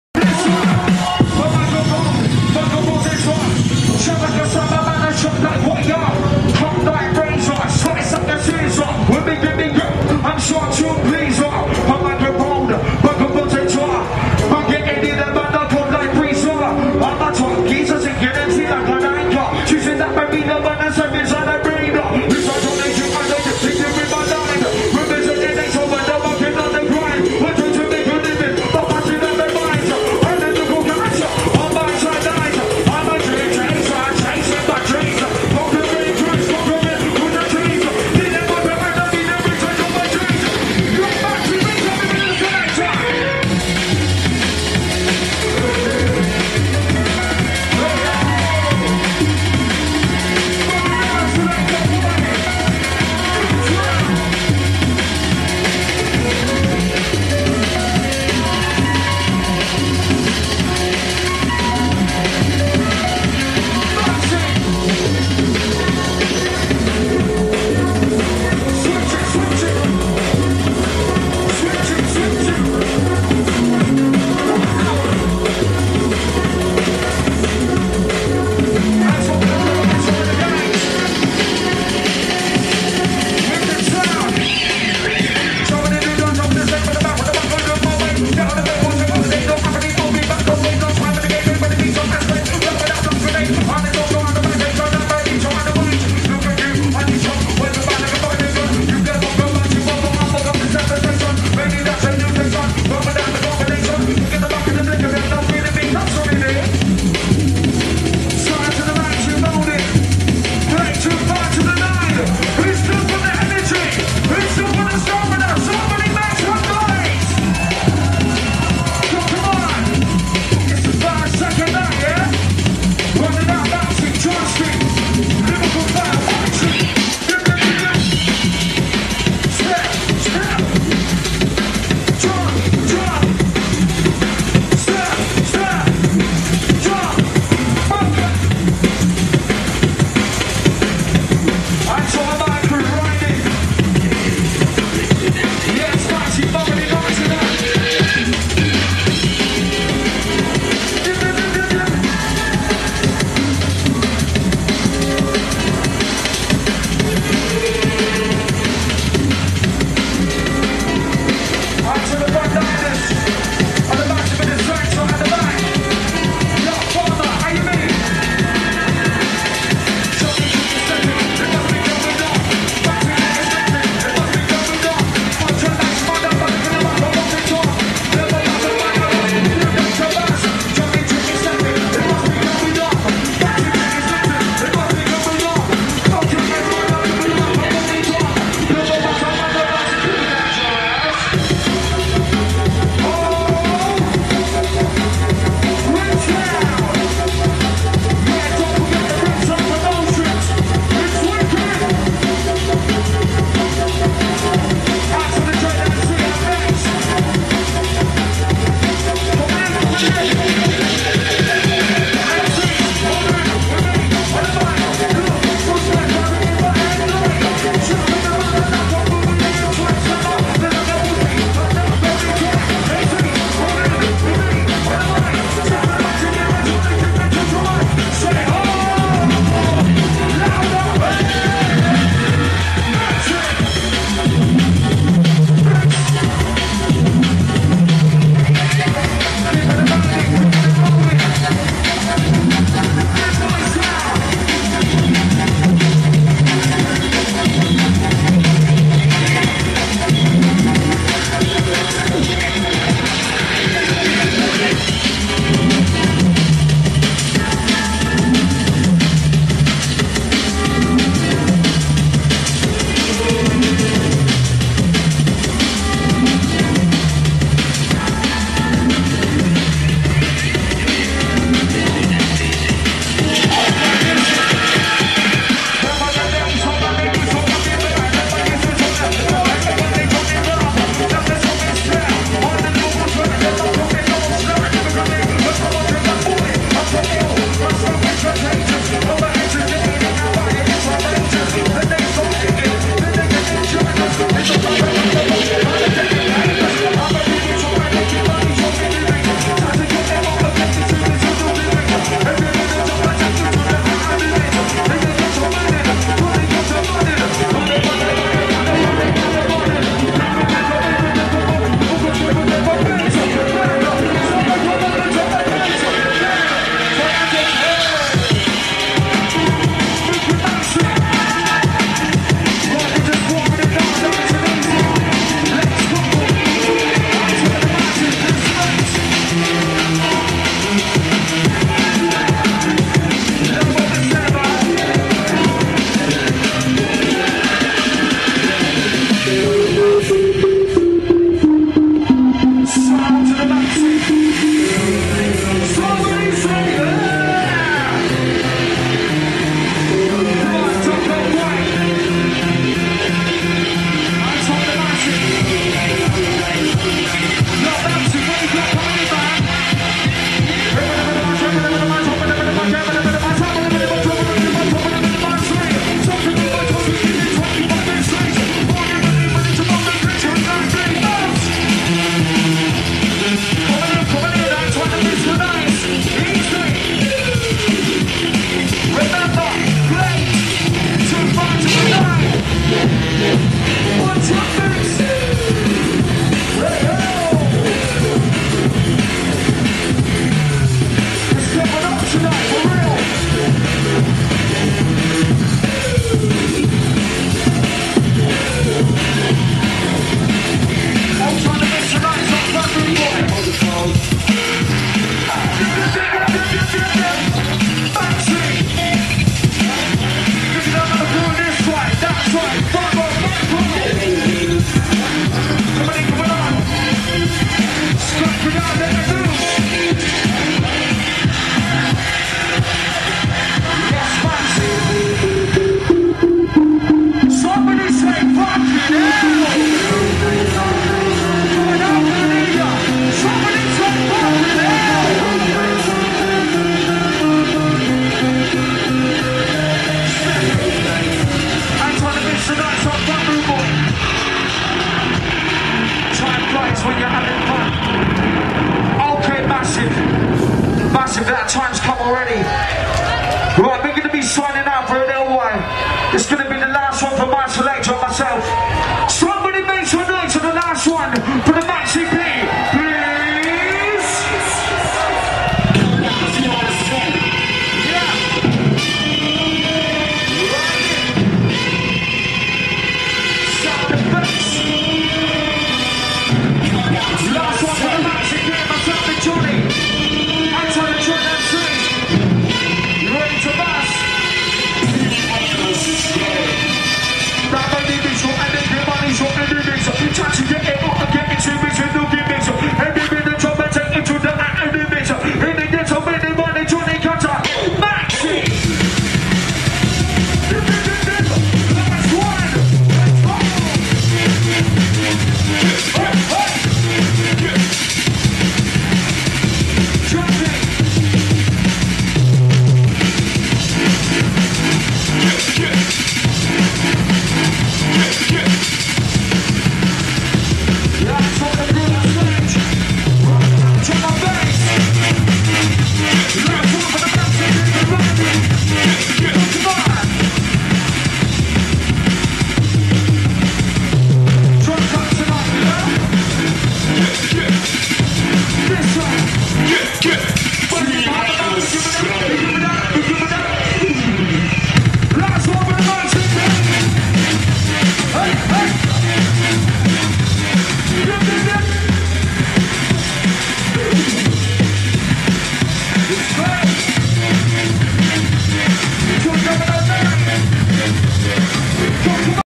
Maxxi P @ Glade Festival 2009 | Part 9 | Breakdown stage
Maxxi P along side Script MC B2B MC Johnny G